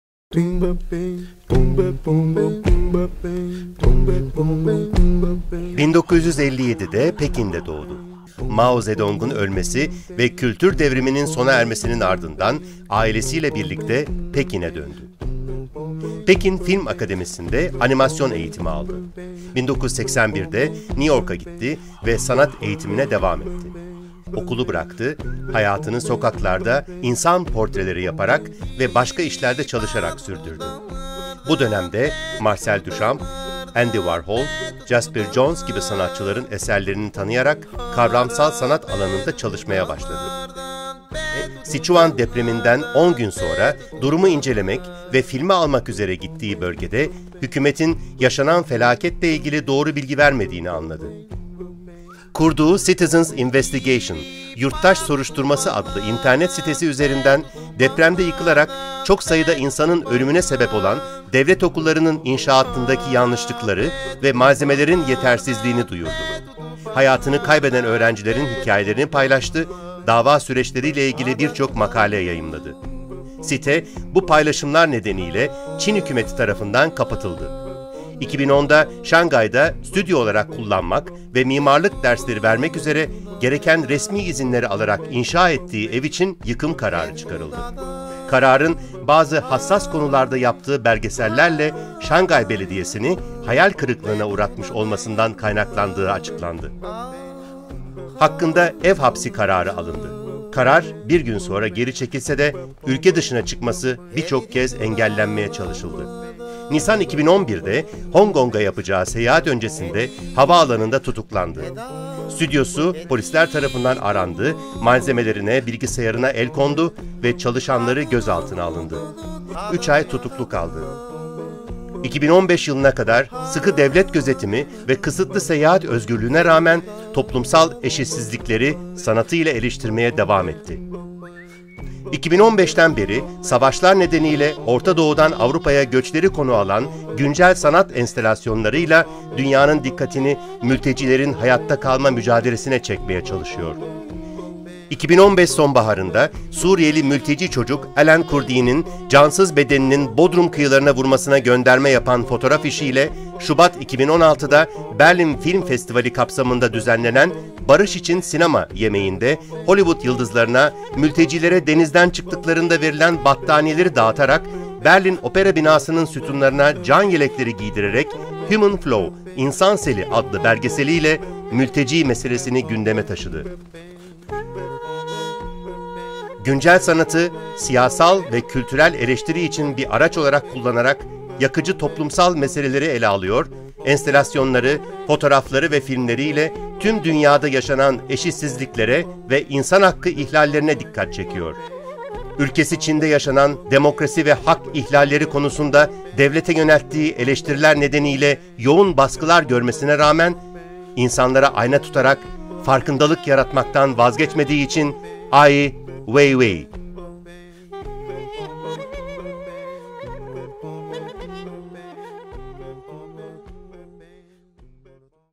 Ai Weiwei - Uluslararası Hrant Dink Ödülü / International Hrant Dink Award / 2017
2017 Uluslararası Hrant Dink Ödülü Çin'den Ai Weiwei'nin

Ai Weiwei 1957’de Pekin’de doğdu. Kültür Devrimi’nin ardından ailesiyle Pekin’e döndü. Pekin Film Akademisi’nde animasyon eğitimi aldı. 1983’de New York’a gitti ve sanat eğitimine devam etti. Ardından okulu bıraktı ve hayatını sokaklarda insan portreleri yaparak ve başka işlerde çalışarak sürdürdü. 1993’te Çin’e döndü. 2008 Siçuan Depremi’nin ardından gittiği bölgede, hükümetin yaşanan felaketle ilgili doğru bilgi vermediğini fark etti. Yıkılan devlet okullarının inşaatındaki hatalara, hayatını kaybeden öğrencilerin hikâyelerine ve dava süreçlerine dair yazılar yayımladığı ‘Citizen’s Investigation’ [Yurttaş Soruşturması] adlı internet sitesi resmî kararla kapatıldı. 2010’da, Şangay’da, stüdyo olarak kullanmak ve mimarlık dersleri vermek üzere inşa ettiği ev için yıkım, kendisi hakkında ise ev hapsi kararı çıkarıldı; mahkûmiyet kararı bir gün sonra geri çekilse de ülke dışına çıkması engellenmeye çalışıldı. 2011’de tutuklandı. Stüdyosu arandı, malzemelerine el kondu, çalışanları gözaltına alındı. Üç ay tutuklu kaldı. 2015’ten beri, Ortadoğu’dan Avrupa’ya göçlere dair enstalasyonlarla dünyanın dikkatini mültecilerin hayatta kalma mücadelesine çekmeye çalışıyor.

The 2017 International Hrant Dink Award goes to Ai Weiwei from China.

Ai Weiwei was born in Beijing in 1957. After the Cultural Revolution, he returned to Beijing. He studied animation at Beijing Film Academy. In 1983 he went to New York to continue his arts education. He left school and made a living by painting portraits on the street. In 1993 he returned to China. In 2008, after the Szechuan Earthquake he visited the region and realized that the government did not provide factual information on the disaster. Creating a ‘Citizens’ Investigation’ website, he released information on faulty construction and sub-standard materials used in schools which caused the death of countless people, shared stories of students who had perished, and published articles about the earthquake during the investigation process. The site was closed by official decree. In 2010 a demolition order was issued for his newly built studio in Shanghai, where he wanted to teach architecture classes. He was ordered to be placed under house arrest; although the order was rescinded the next day, it was followed by attempts to prevent him from leaving the country. In 2011 he was apprehended. His studio was searched, materials confiscated, co-workers detained. He was held for three months. Since 2015 he uses his art installations on the flight from Middle East to Europe, to draw the world’s attention to refugees’ struggle to survive.